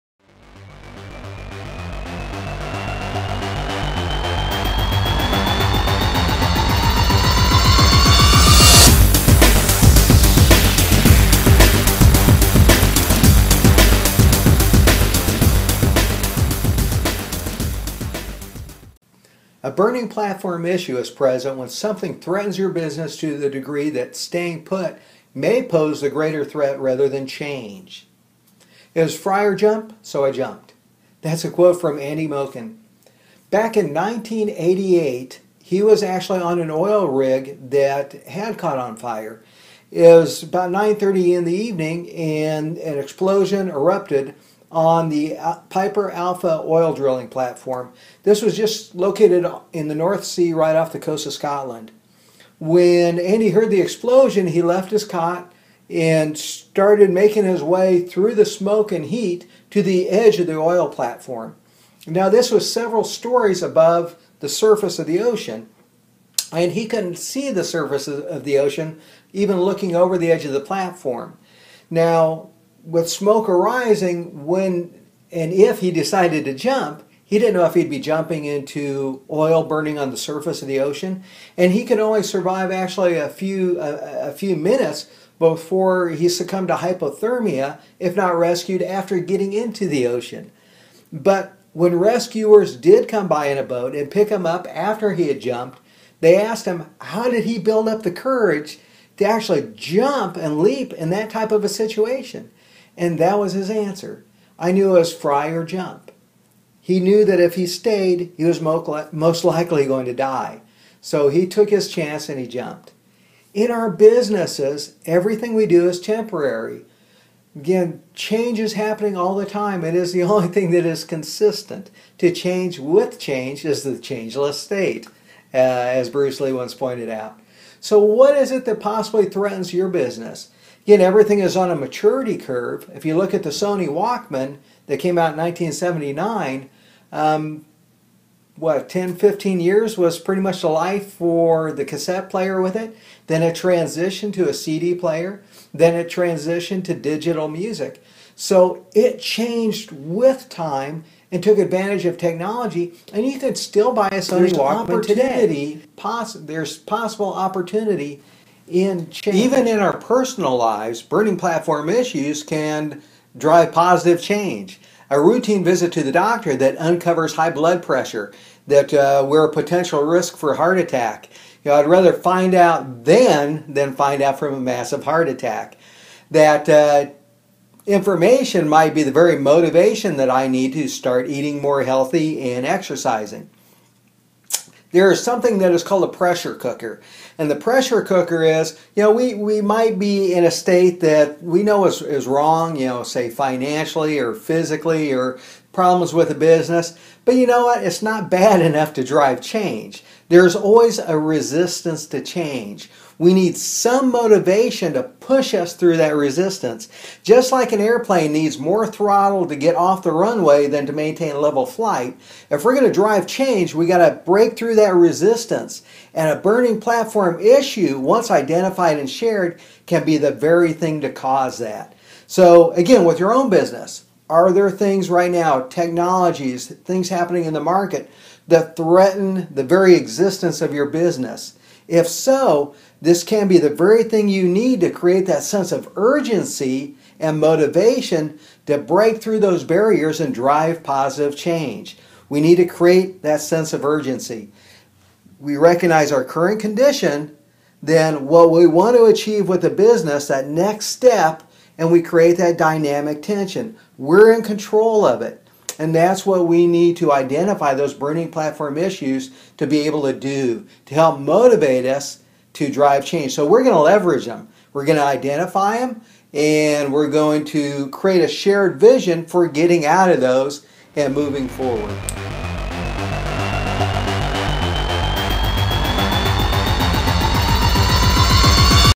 Burning Platform issues
A burning platform issue can be defined as a problem that is so impactful that the very existence of the company may be threatened.

Once again I remind you that everything we do in life is temporary. Today’s great breakthrough achievements could be the very thing that holds us back tomorrow.

A burning platform scenario surfaces when stress is being raised over maintaining the current condition. Knowledge may come to light of a threat that forces change. We see that to stay as is may be a greater threat than to risk changing.